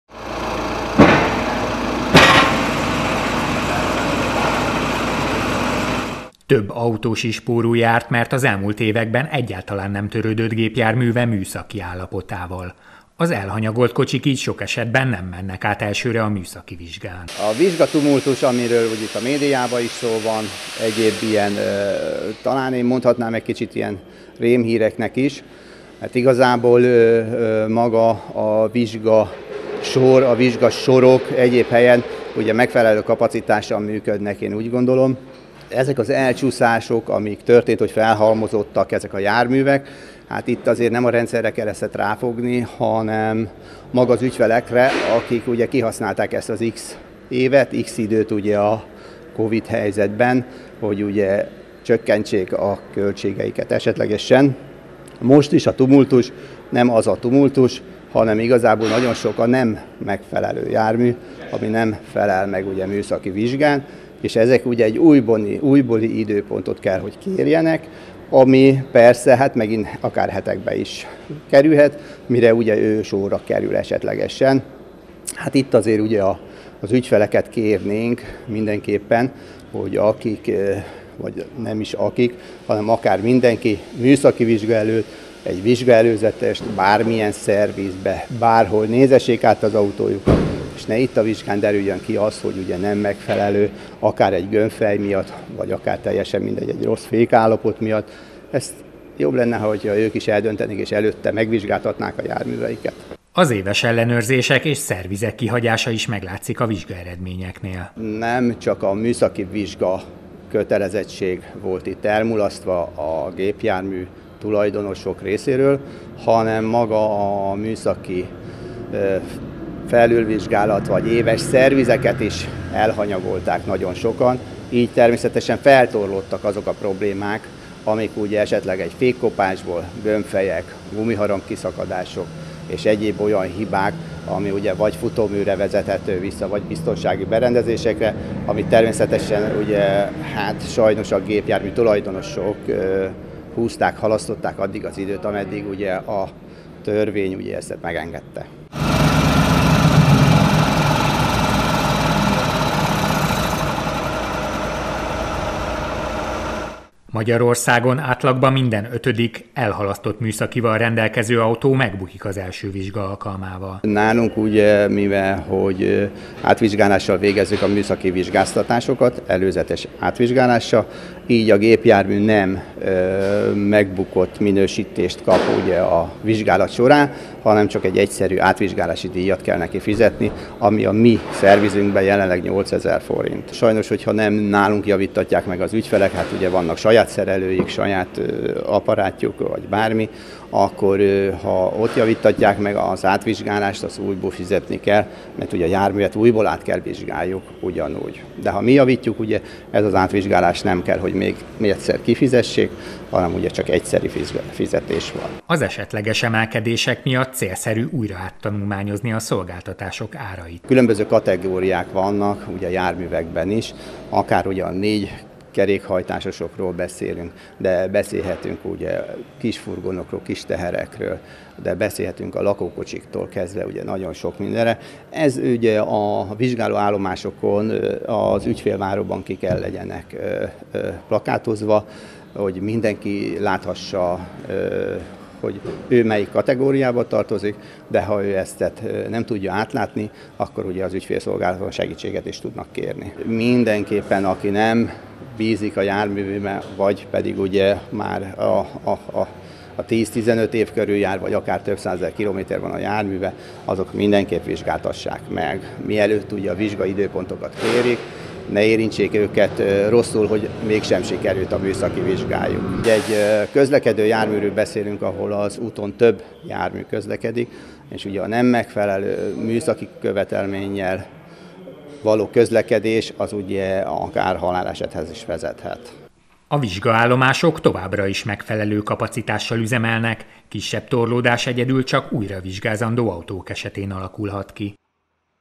A nem megfelelően karbantartott autók megbuknak a kötelező műszaki vizsgán
Sokan próbáltak meg spórolni a türelmi rendelet alatt, ám annak határideje június 30-án véget ért. Eddig kaptak ugyanis engedményt azok az tulajdonosok, akik gépjárművének műszakija a pandémia ideje alatt járt le. A szakember véleménye szerint ezek az autósok most rosszabbul járhatnak, mert több évnyi javítás költsége is a nyakukba szakadhat.